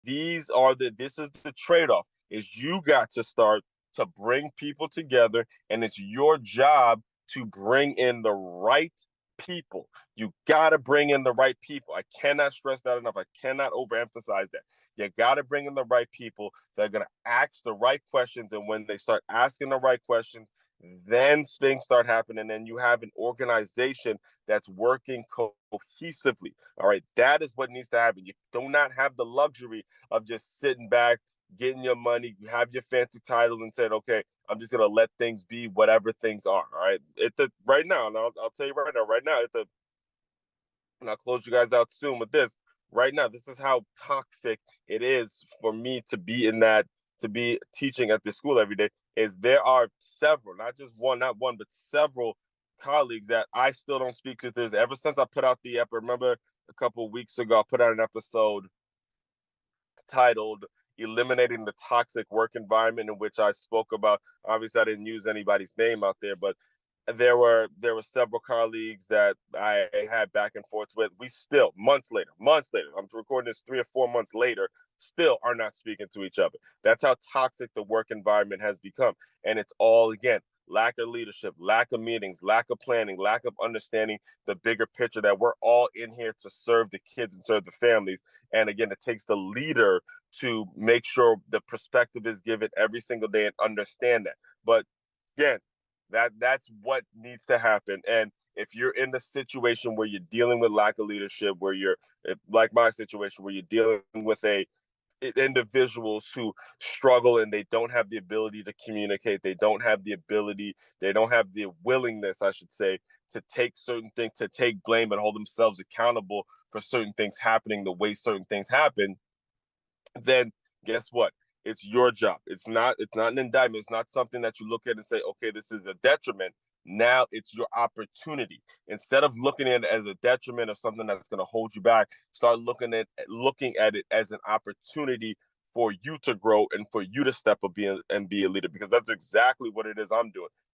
How to Build a Cohesive Organization by Bringing in the Right People
In this video, we discuss the importance of bringing in the right people to create a cohesive organization. Learn why it is crucial to ask the right questions and foster a positive work environment.